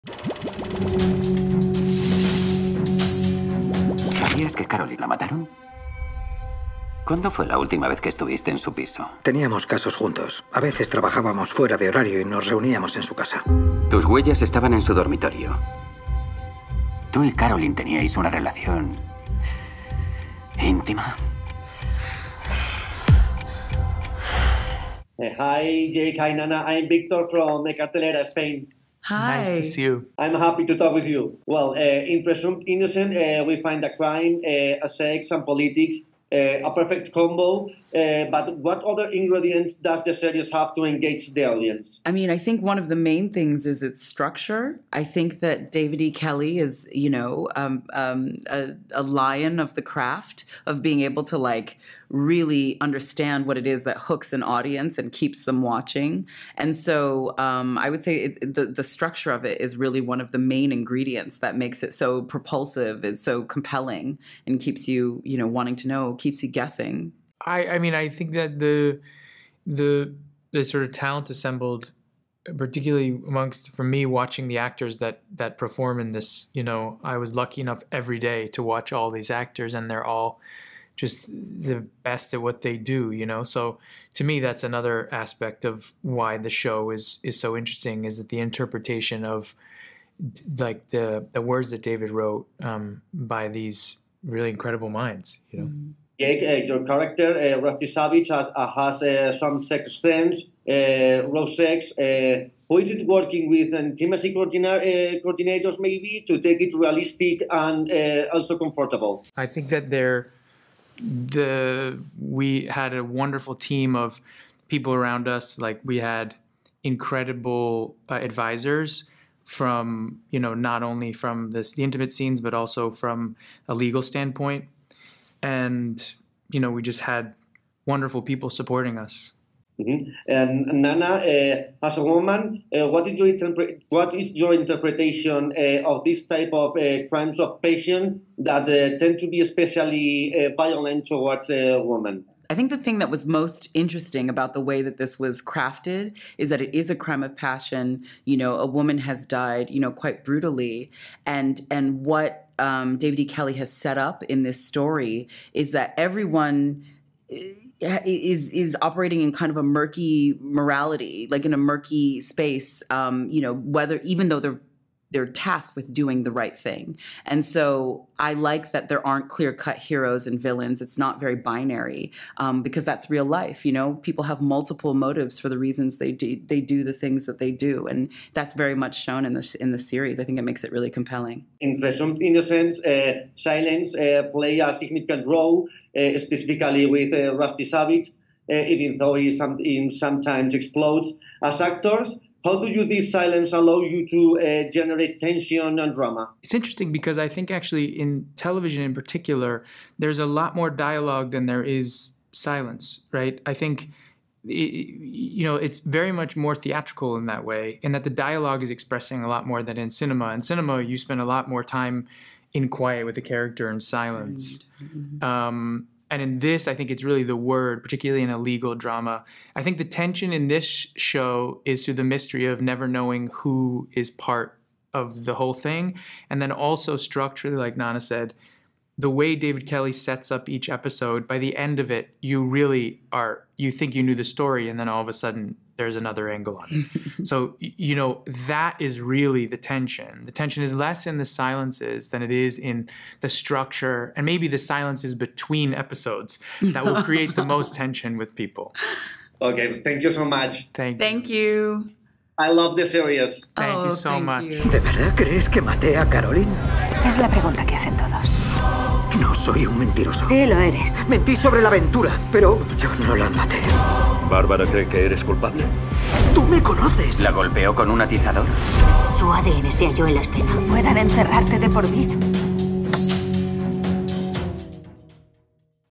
Jake Gyllenhaal: "Cuando en 'PRESUNTO INOCENTE' piensas que lo tienes claro, cambia el enfoque"
Hablamos con Jake Gyllenhaal y Nana Mensah, quienes interpretan a Rusty Sabich y Alana Rodriguez en 'Presunto Inocente', serie de Apple TV+ creada por David E. Kelly basada en la novela de 1987 del mismo nombre de Scott Turow sobre un crimen pasional en el seno de la Fiscalía.